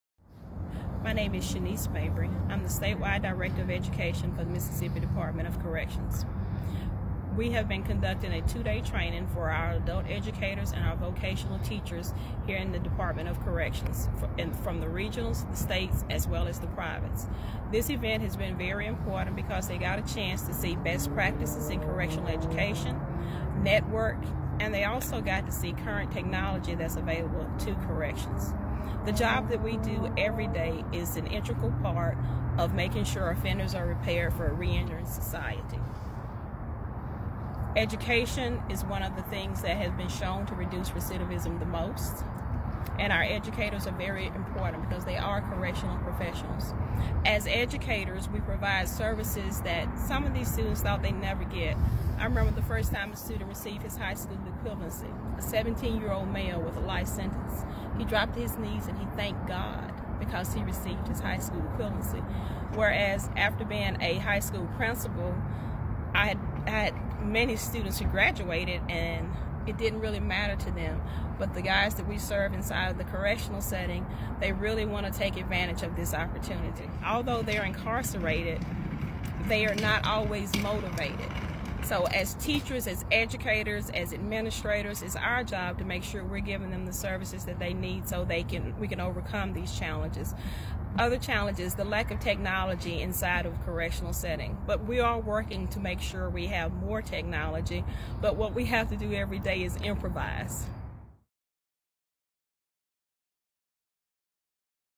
Correctional Educators in the MDOC System
MDOC Director of Education Shaniece Mabry talks about the importance of correctional educators and the recent conference held in Jackson for them to share best practices, network, and learn about new technology and products.